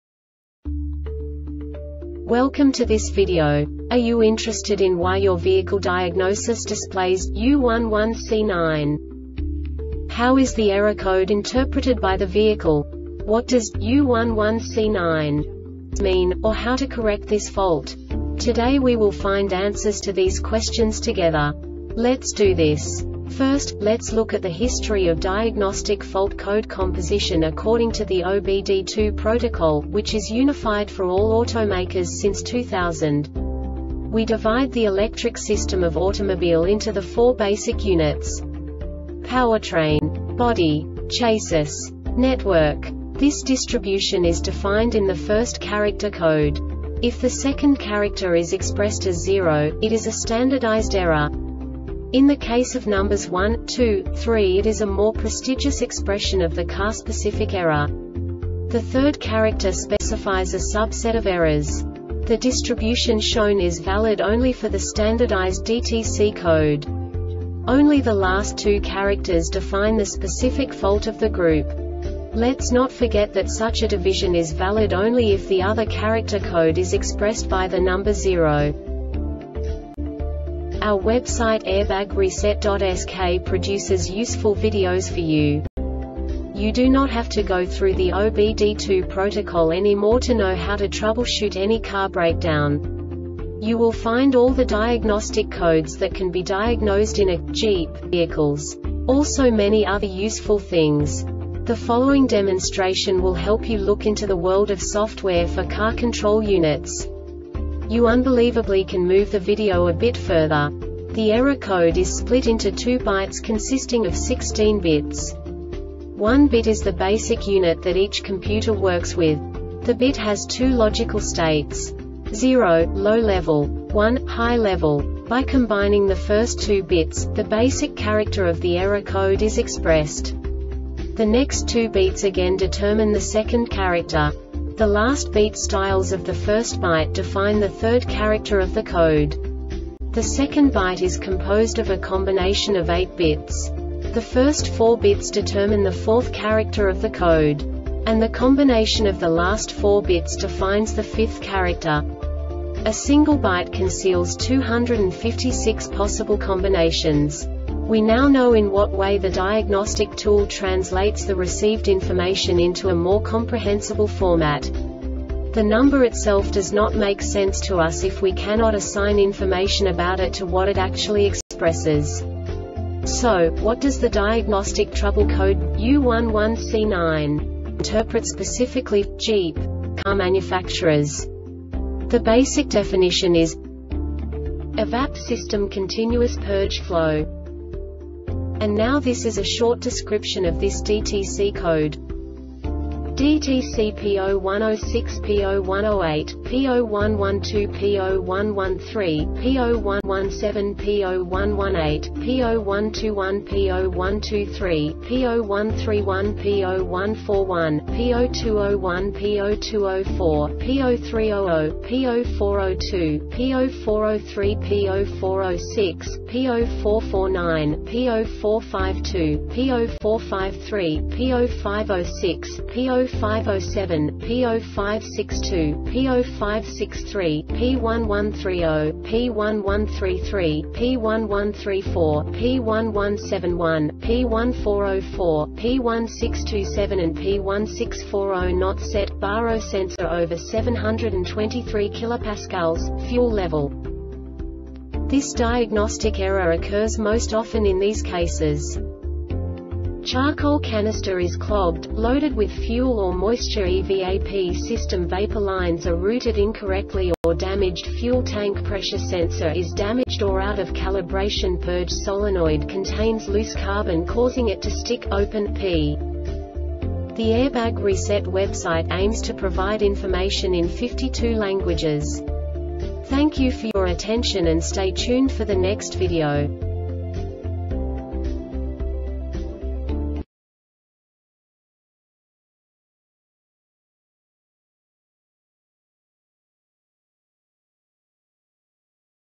DTC Jeep U11C9 Short Explanation
Contents:
0:21 Basic DTC analysis according to OBD2 protocol standard.
1:48 Insight into programming
2:58 A diagnostically understandable description of the Diagnostic Trouble Code
3:05 Basic error definition

Make:
Jeep
Code:
U11C9
Definition:
EVAP System Continuous Purge Flow
Description:
Cause:
Charcoal canister is clogged,loaded with fuel or moistureEVAP system vapor lines are routed incorrectly or damagedFuel tank pressure sensor is damaged or out-of-calibrationPurge solenoid contains loose carbon causing it to stick "open"P